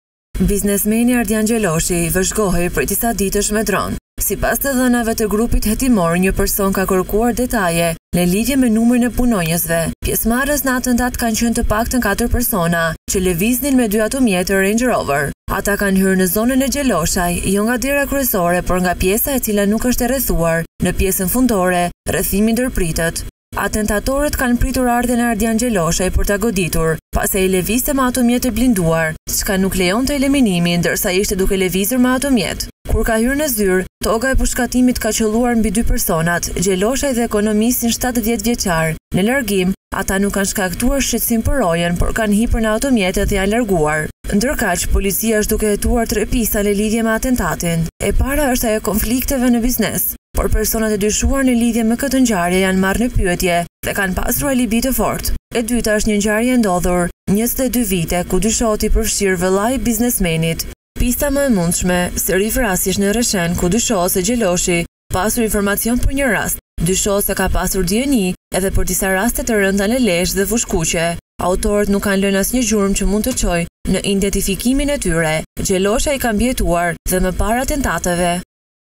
Atentati në Kurbin/ Ja si dështuan vrasësit me pagesë
Biznesmeni Ardjan Gjeloshi vëzhgohej prej disa ditësh me dron.
Sipas të dhënave të grupit hetimor, një person ka kërkuar detaje në lidhje me numrin e punonjësve.
Pjesëmarrës në atentat kanë qenë të paktën 4 persona që lëviznin me 2 automjete Range Rover.
Ata kanë hyrë në zyrën e Gjeloshaj, jo nga dera kryesore por nga një pjesë e cila nuk është e rrethuar. Në pjesën fundore rrethimi ndërpritet.
Atentatorët kanë pritur ardhjen e Ardjan Gjeloshaj për ta goditur, pasi ai lëvizte me automjet të blinduar, çka nuk lejonte eliminimin ndërsa ishte duke lëvizur me automjet.

𝐀𝐮𝐭𝐨𝐫 : NTV